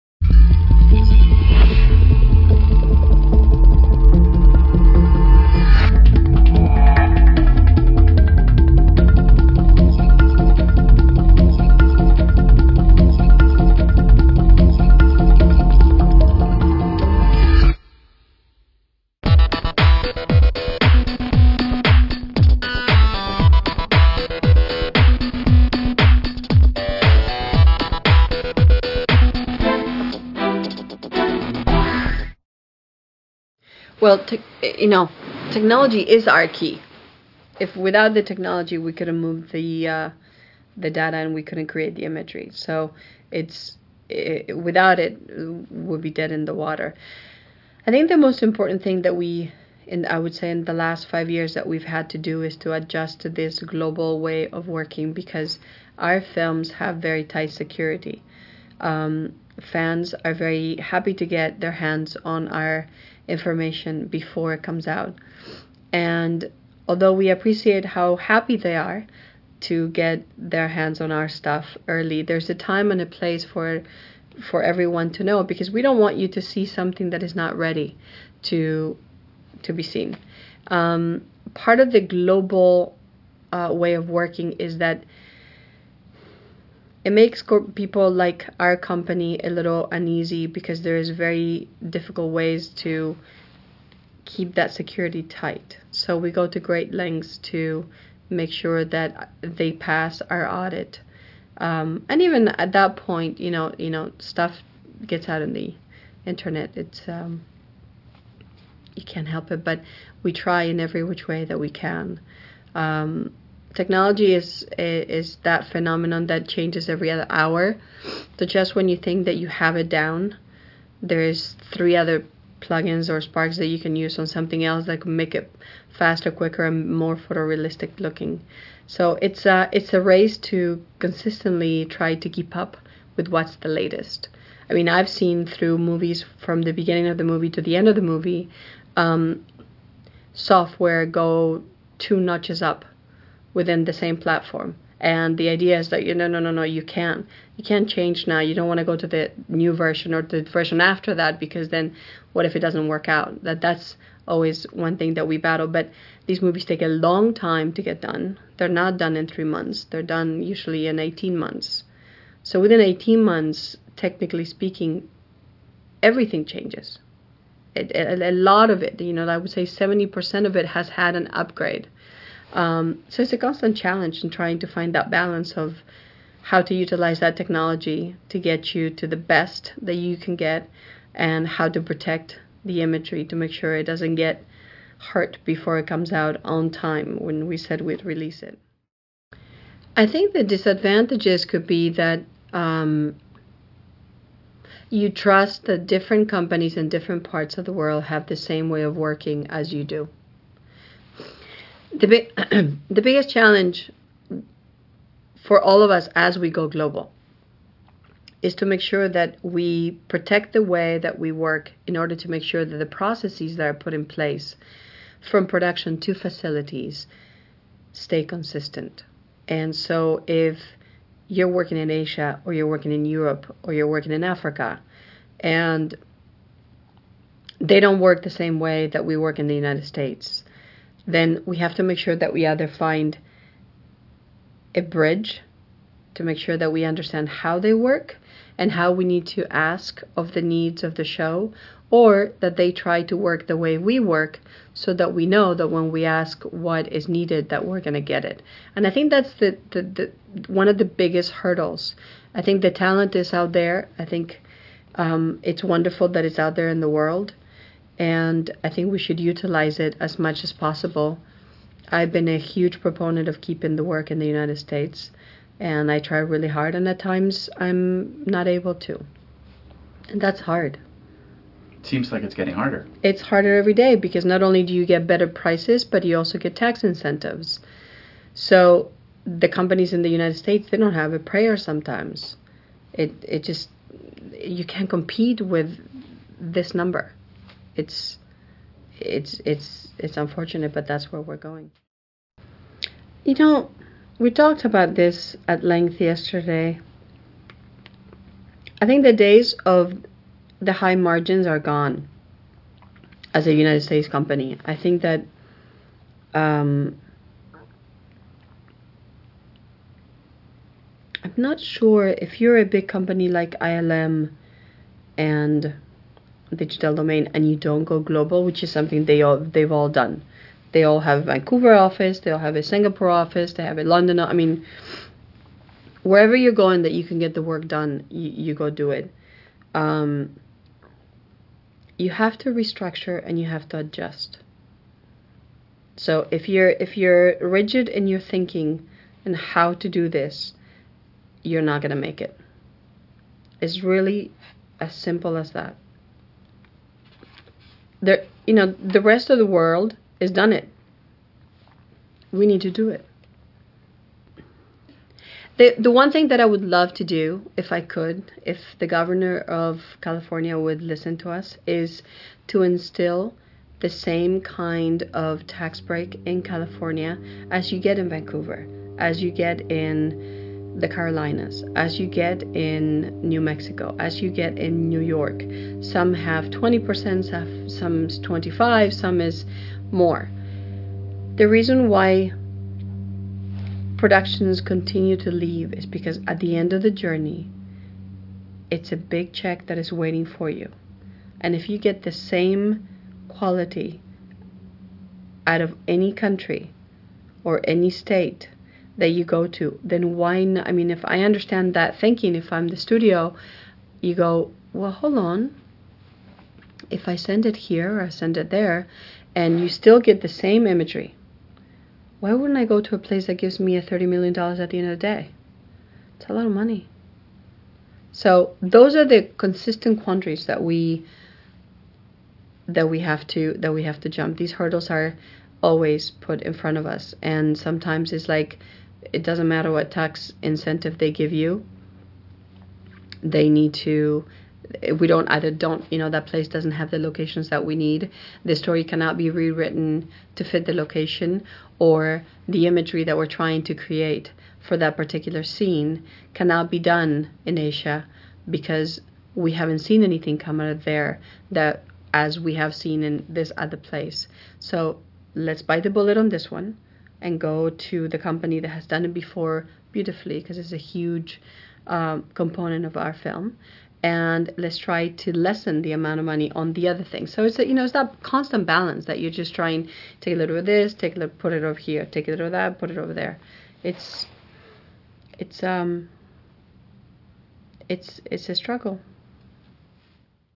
Interview with Victoria Alonso executive vice president VFX at Marvel Studios (Part 2 of 3)
Victoria discusses the importance of technology and a global pipeline for Marvel film production